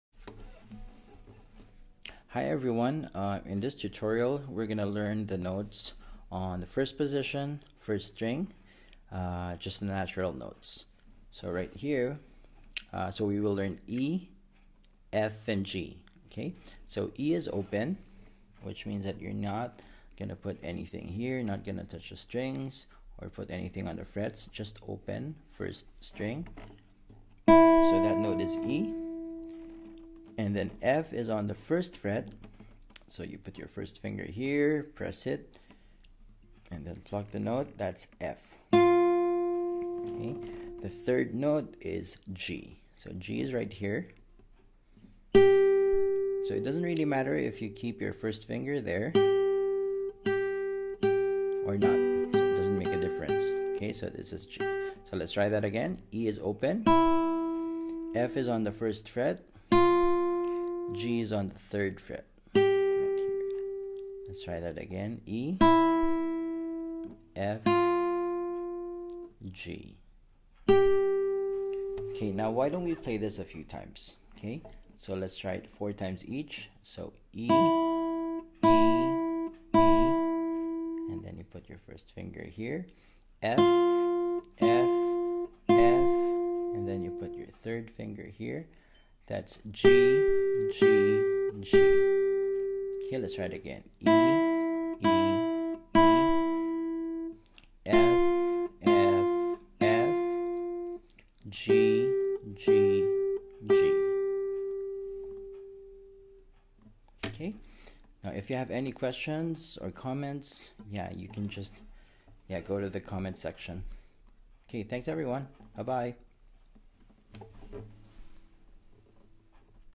Notes on the 1st String E - F -G
This lesson explains the 1st postion natural notes on the 1st string
E - open 1string
F - 1st Fret, 1st finger
G -  3rd fret, 3rd finger